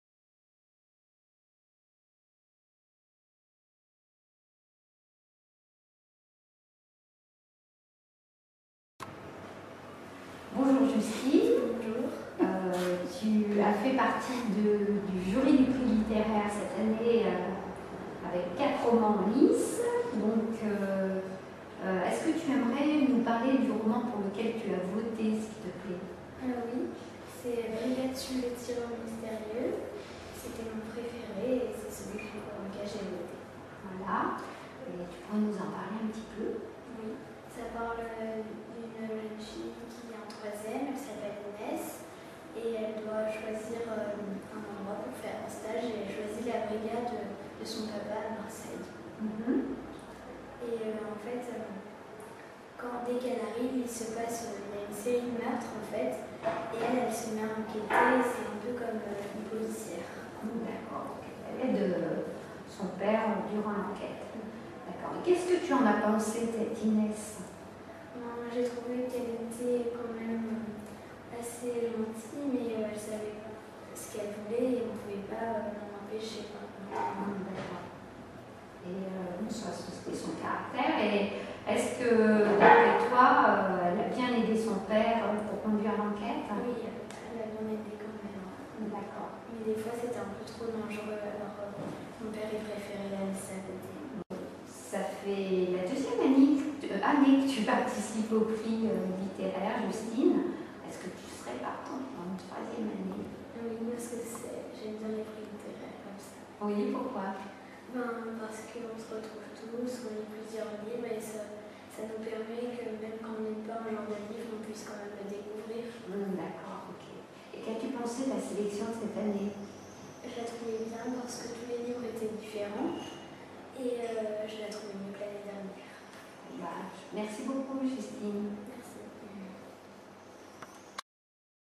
Prix littéraire jeunesse 2012. Le coup de coeur de Justine
Munis de leur carte d'électeur spécial Prix littéraire, tous les élèves des 8 classes de CM/6ème sont passés dans l'isoloir avant de déposer leur bulletin dans l'urne. En fin de journée, les 26 lecteurs individuels inscrits au Prix ont voté à leur tour. Justine partage avec nous son coup de cœur.